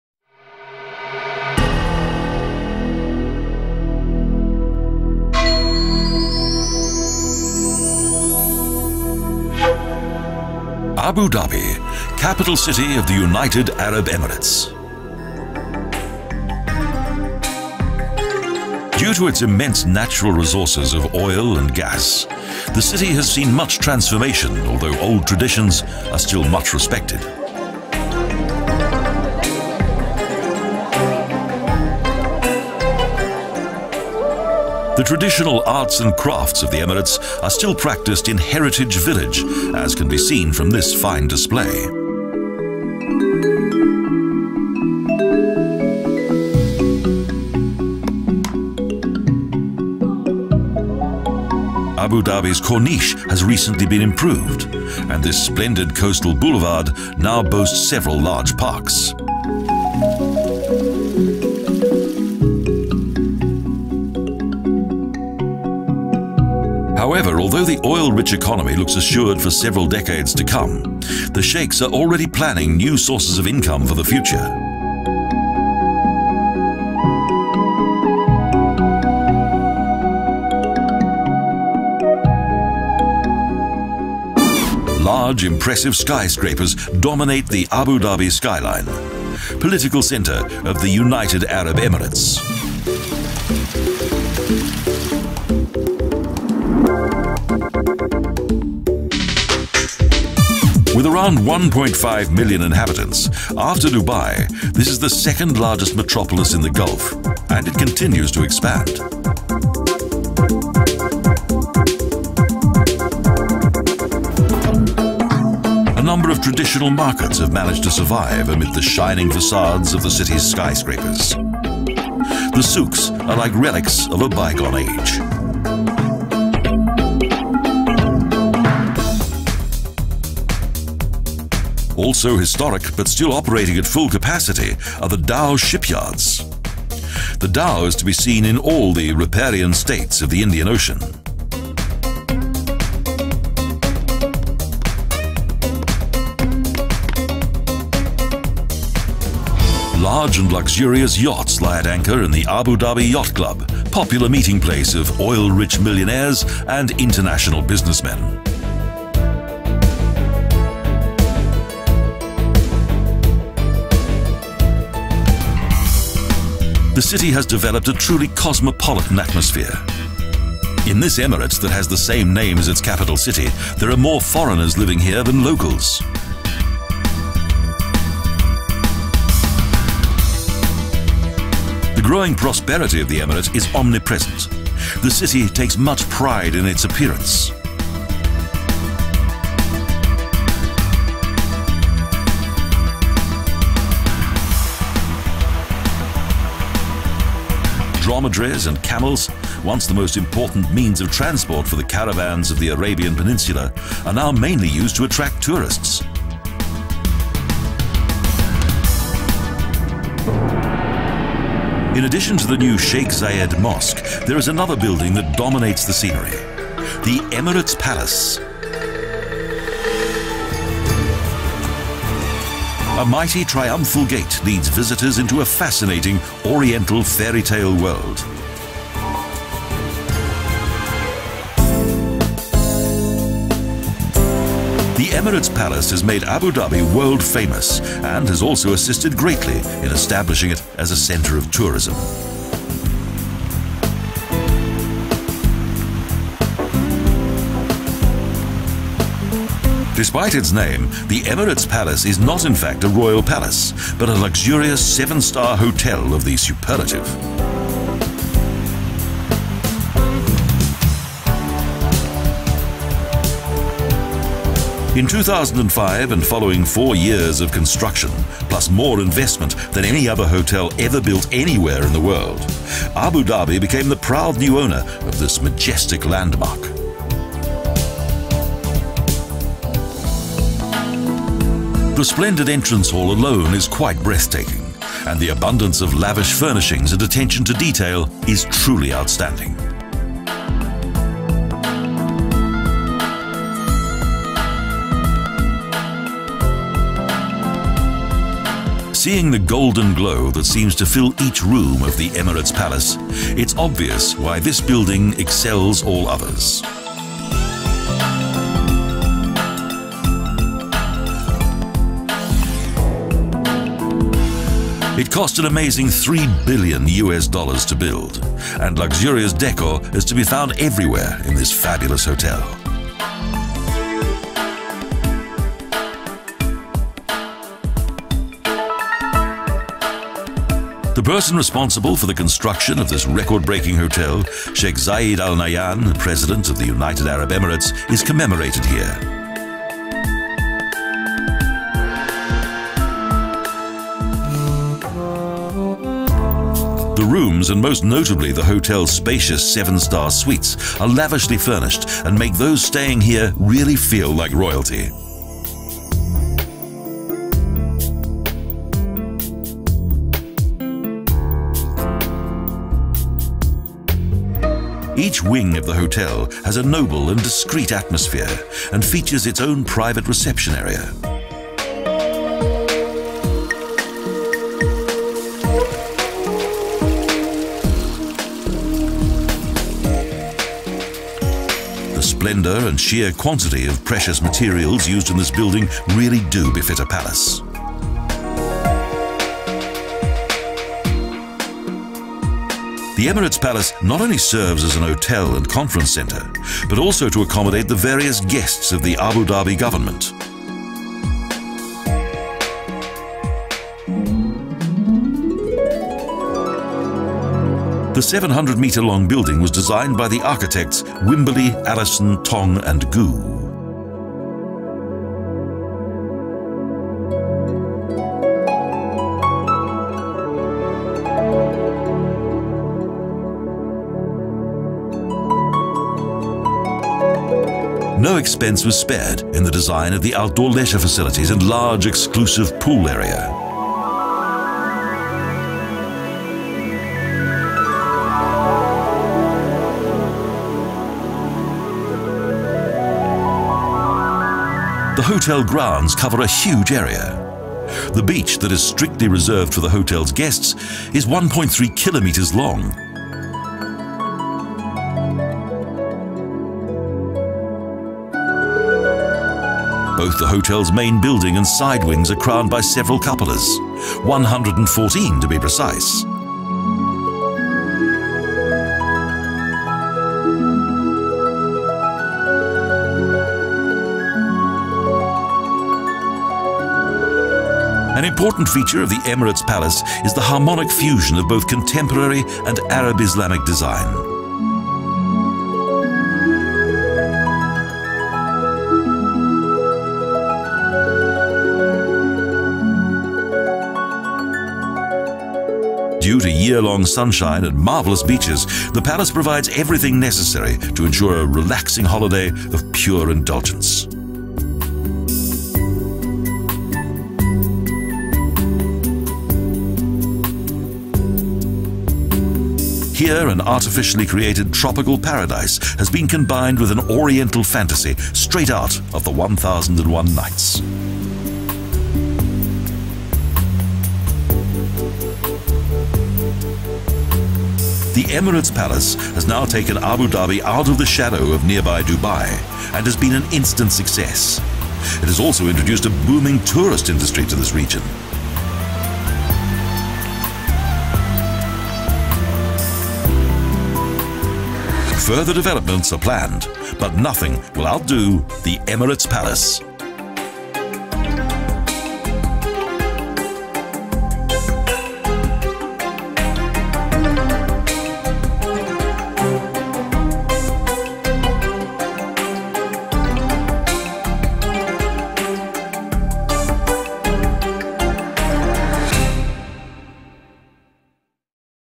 Emirates Palace Vacation Travel Video Guide
Modern time wonder Emirates Palace in Abu Dhabi.
Abu Dhabi is capital city of the United Arab Emirates and due to its immense natural resources of oil and gas has seen much transformation, although age-old traditions are still much respected. Even so, the city has developed a truly cosmopolitan atmosphere. and contains more foreigners than locals. A mighty triumphal gate leads visitors into a fascinating Oriental fairytale world, the Emirates Palace, which has made Abu Dhabi world famous and has also assisted greatly in establishing it as a centre of tourism. Despite its name the Emirates Palace is not in fact a royal palace but a luxurious seven star hotel of the superlative and, at three billion U.S. dollars, cost more to build than any other hotel ever built anywhere in the world. Both the hotel’s main building and side wings are crowned by several cupolas, a hundred and fourteen to be precise! The Emirates Palace has taken Abu Dhabi out of the shadow of nearby Dubai and although further developments are planned nothing will outdo the Emirates Palace!